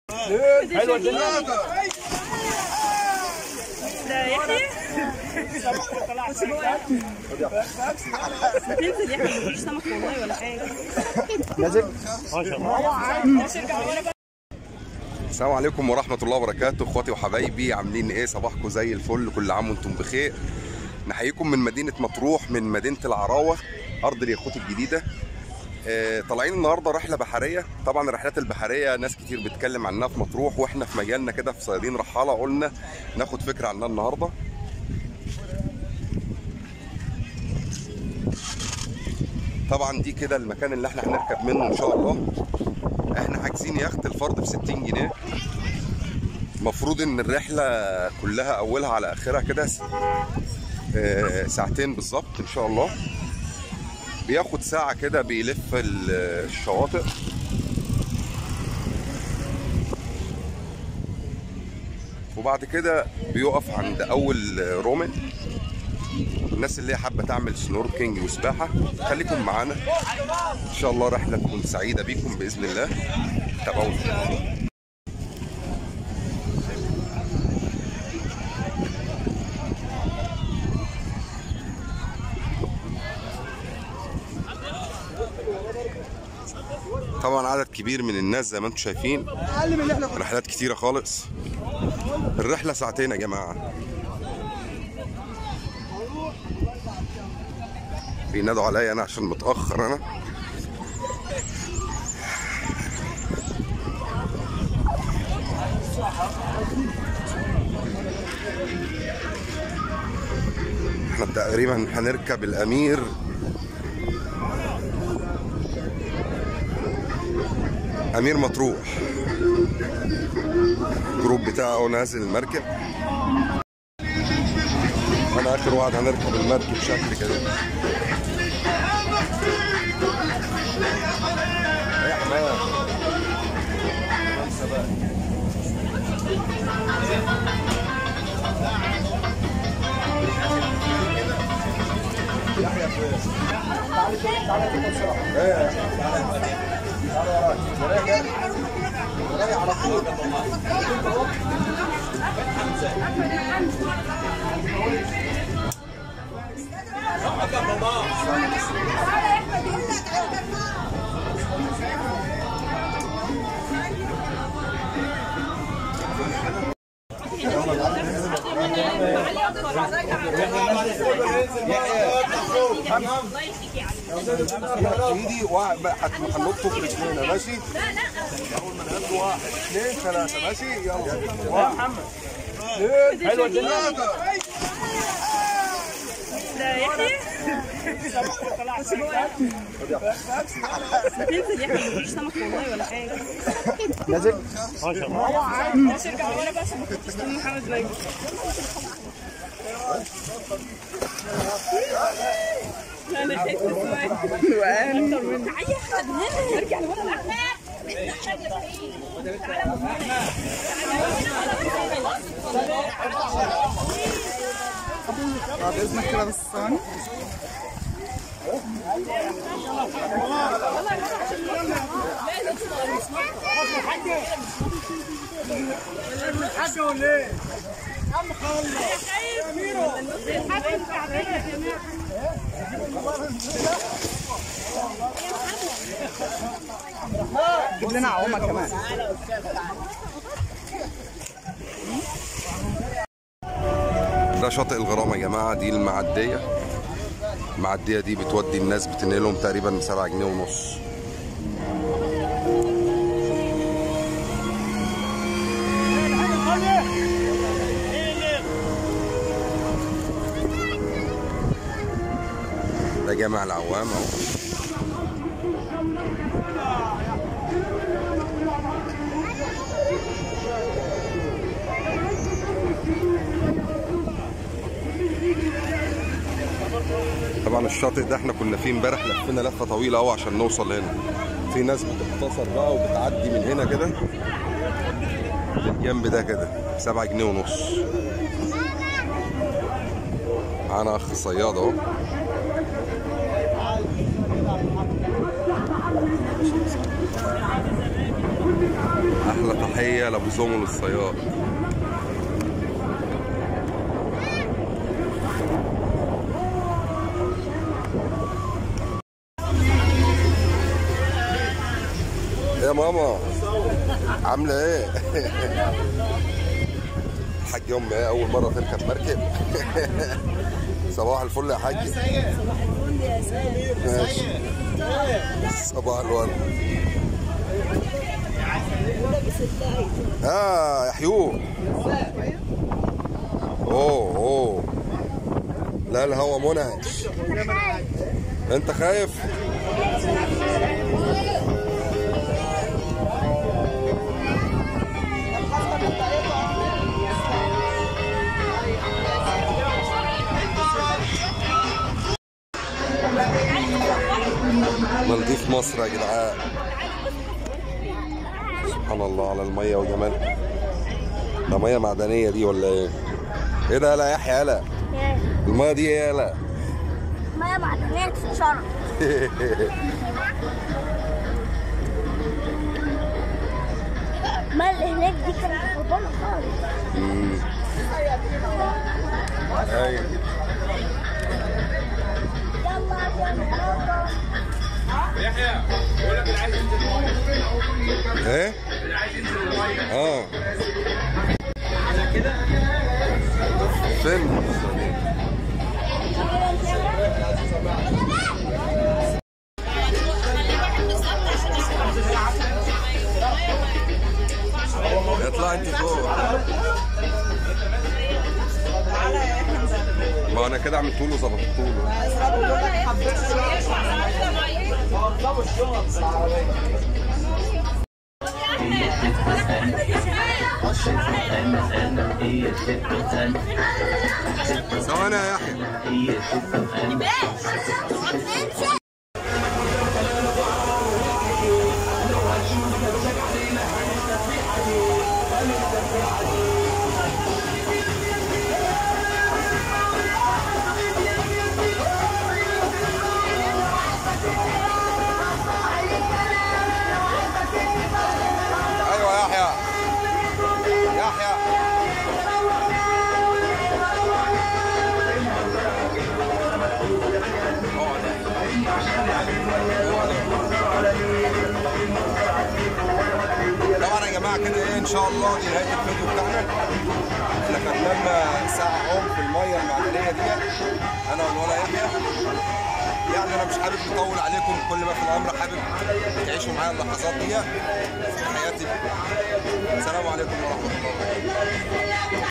تجربة لرحلة باليخت في مرسى مطروح
رحلات اليخت من مطروح بكل تفاصيلها الرحلة البحرية اللي كل زوار مطروح بيحكوا عليها